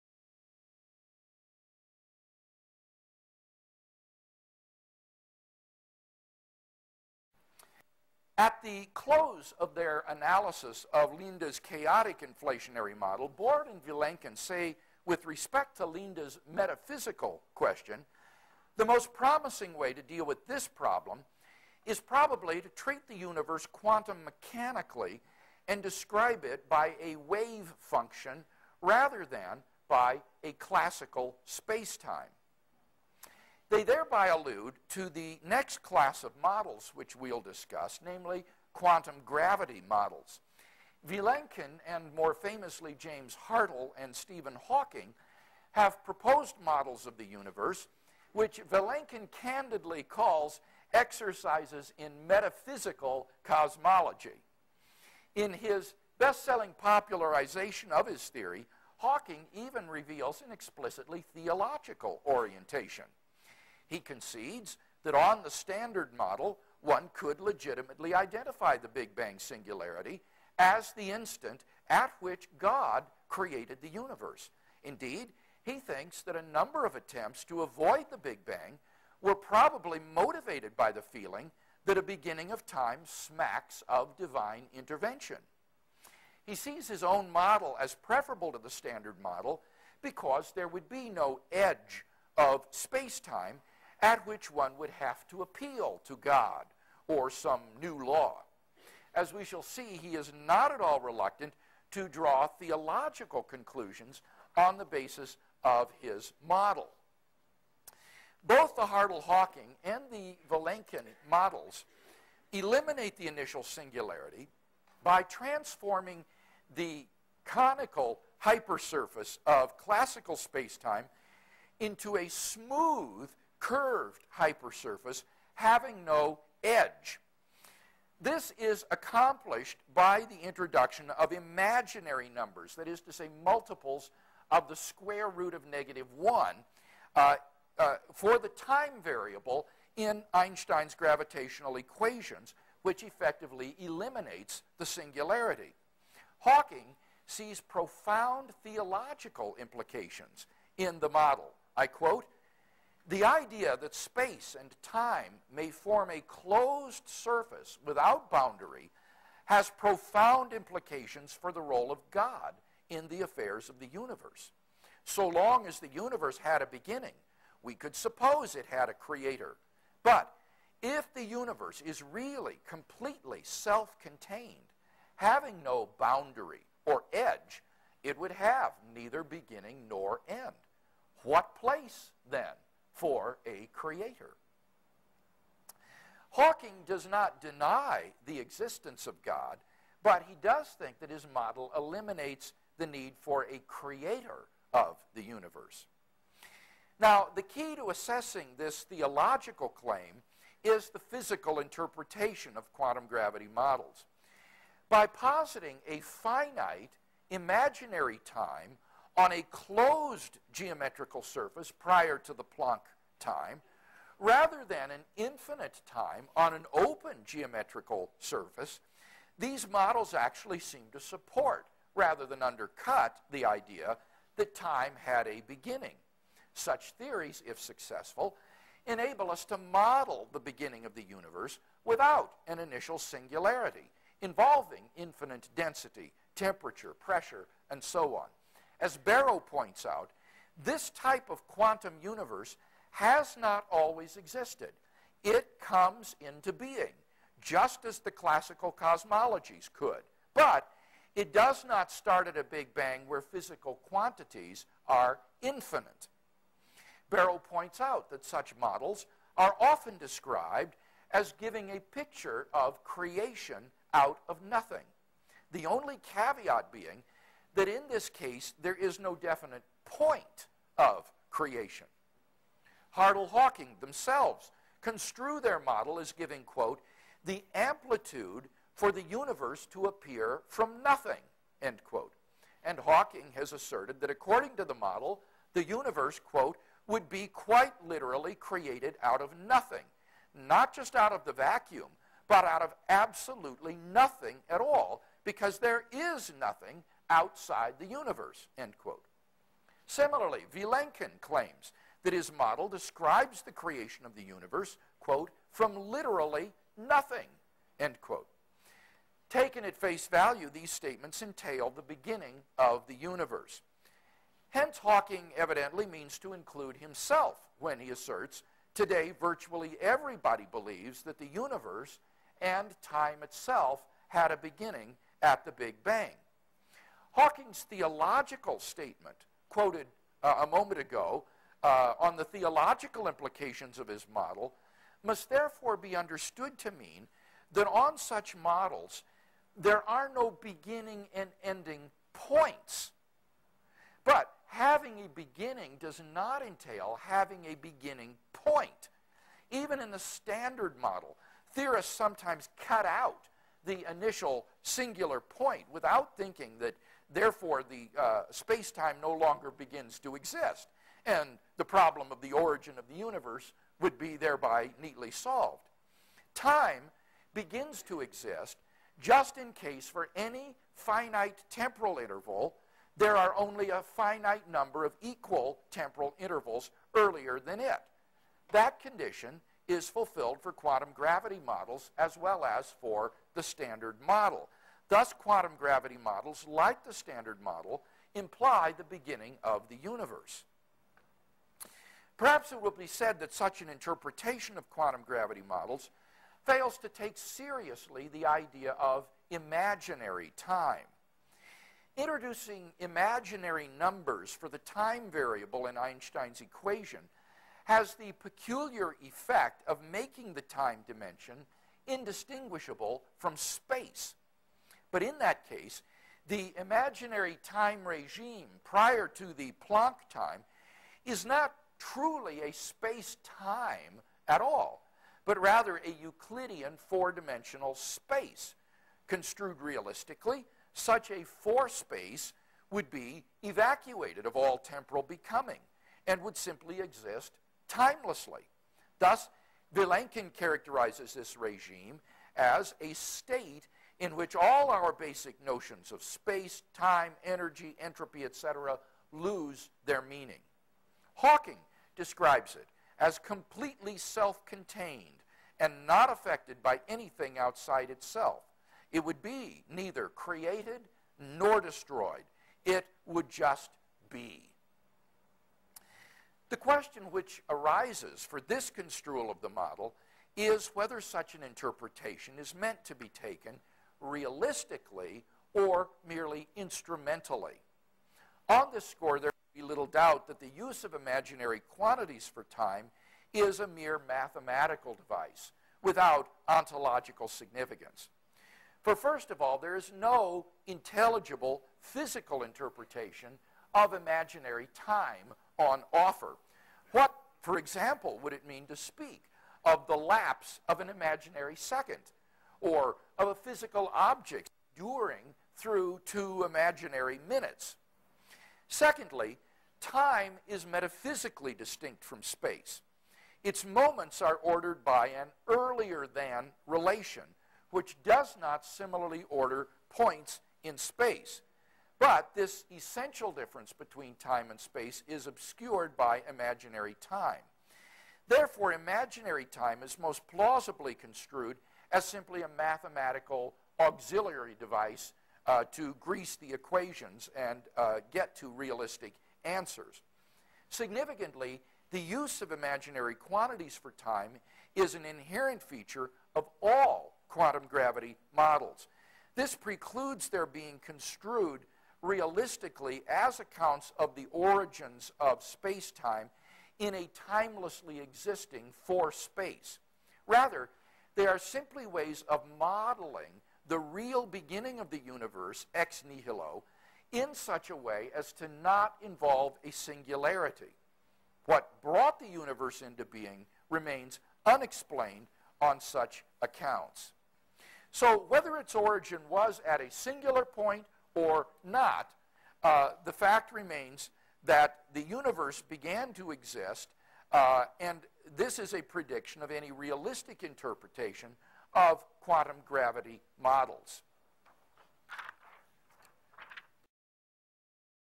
Quantum Gravity Theory has Problems - William Lane Craig, PhD
William Lane Craig speaks on the Quantum Gravity Model (via James Hartle and Stephen Hawking) and the beginning of the universe. Does the Hartle-Hawking Model show that God did not create the universe?